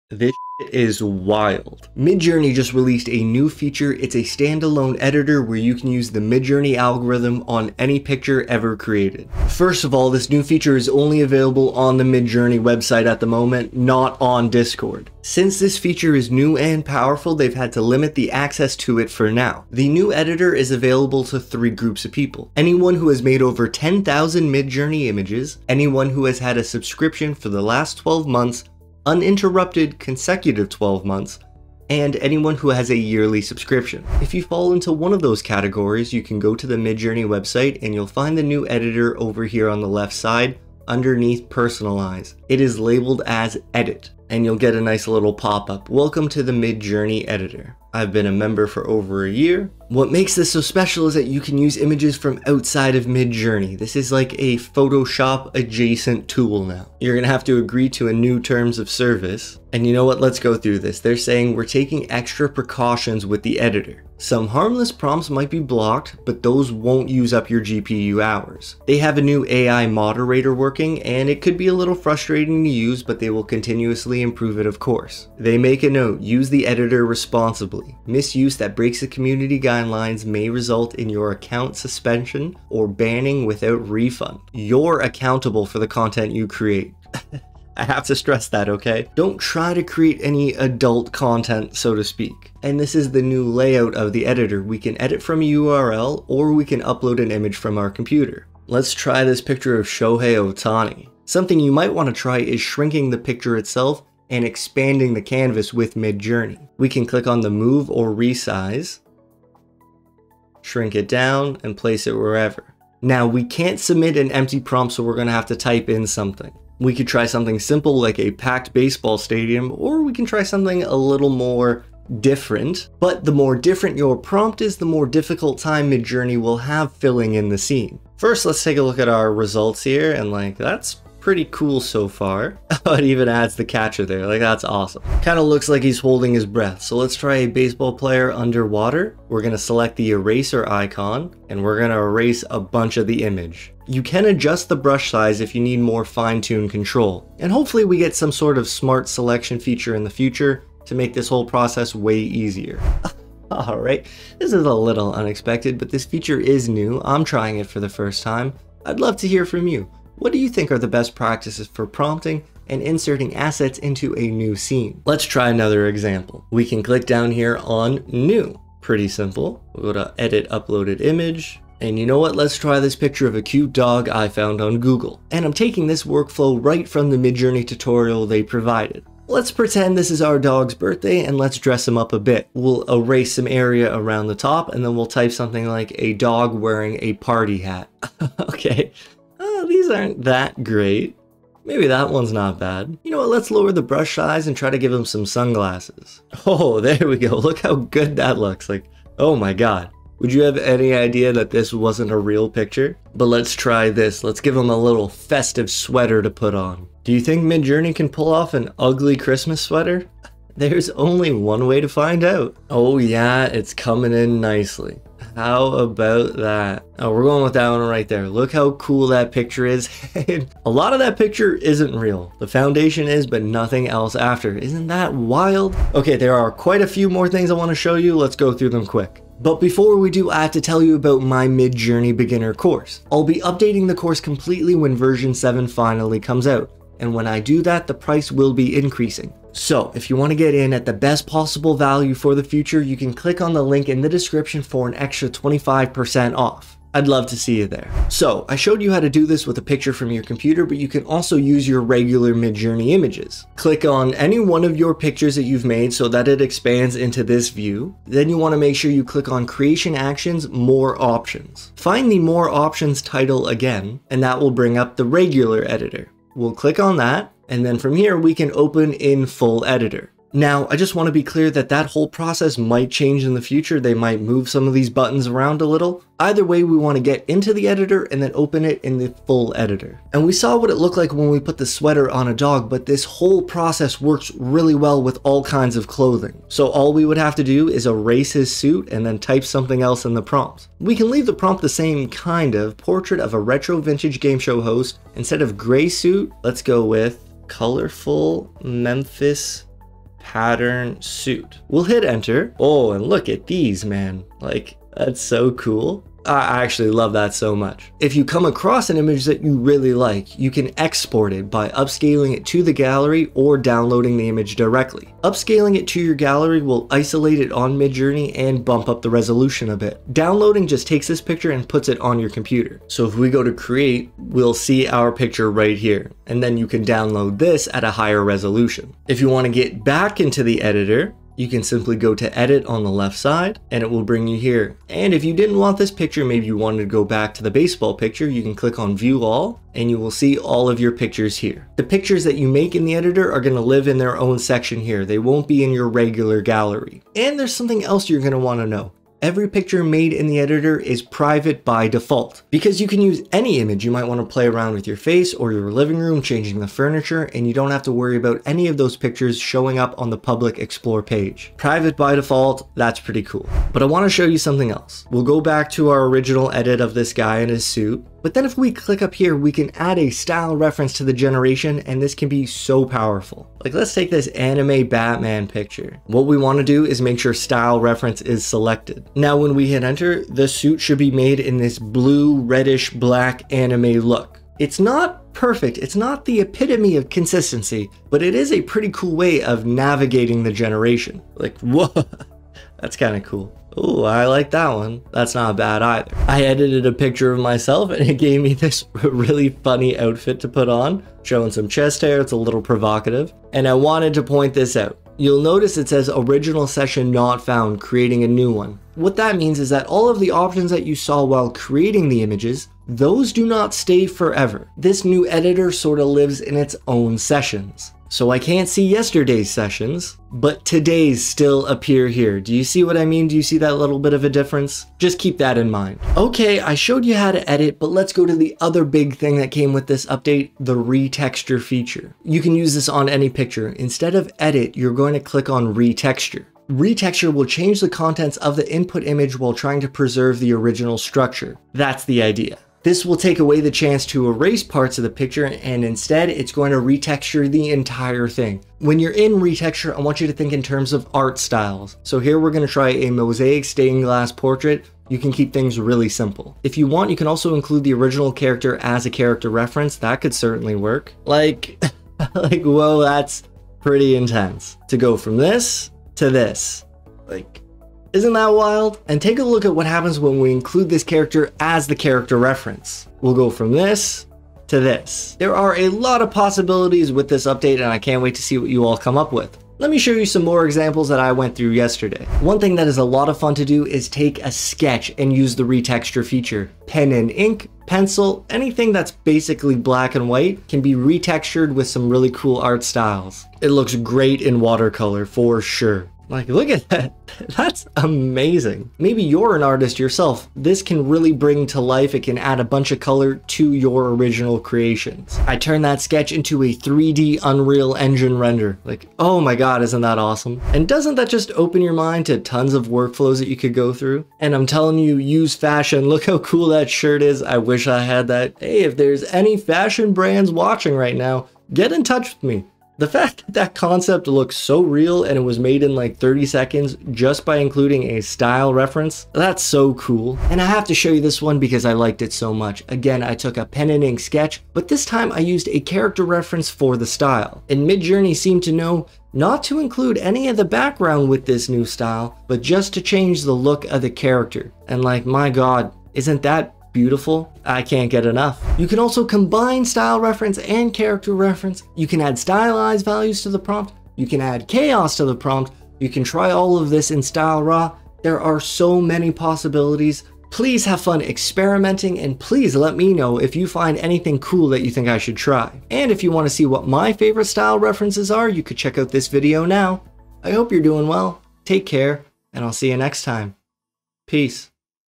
BIG Midjourney UPDATE! Edit ANY Image
Timestamps:
0:00 Intro
0:19 How to Access
0:44 Warning
1:32 Edit ANY Image
2:45 Example 2
4:08 Edit Midjourney Images
5:07 Export / Download
6:03 Style Reference
6:36 'Sessions' Explained
7:06 RETEXTURE
7:59 Fun Thing to Try